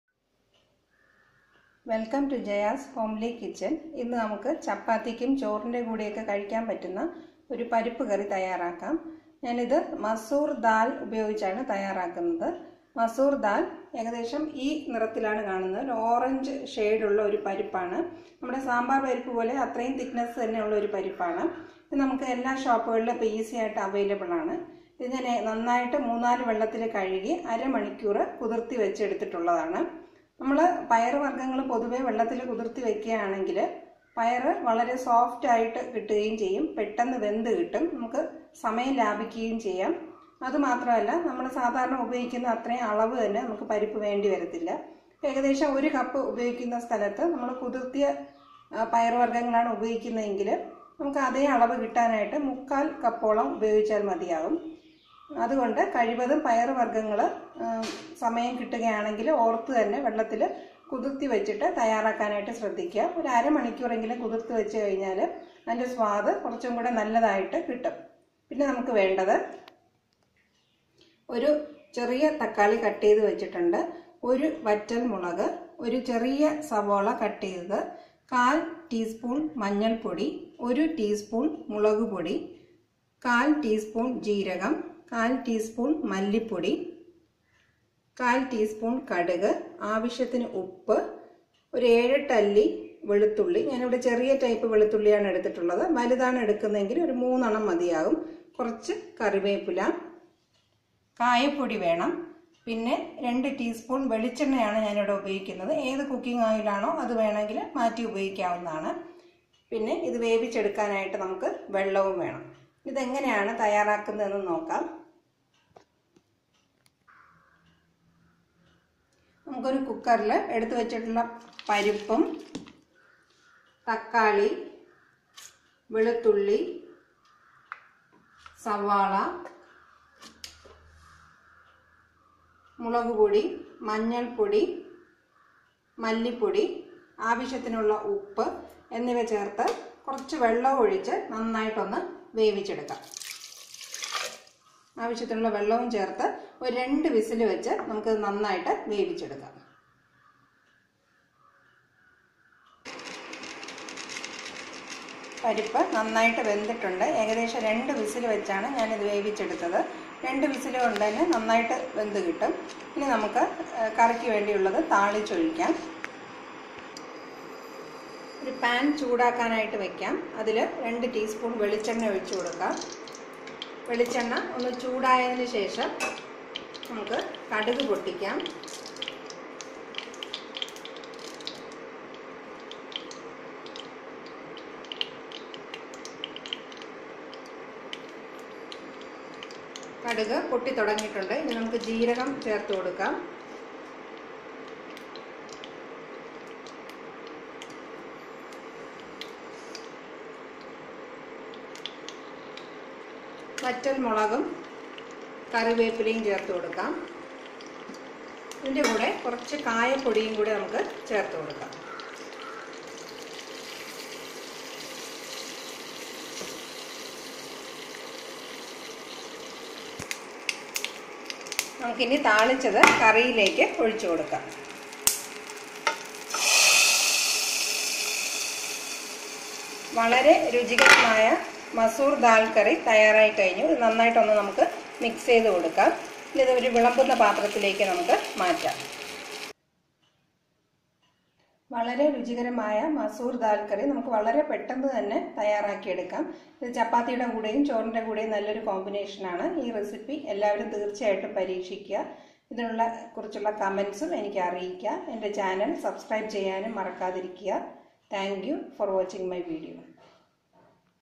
masoor dal curry (malayalam)
A delicious and nutritious curry which goes well with chappathi and rice.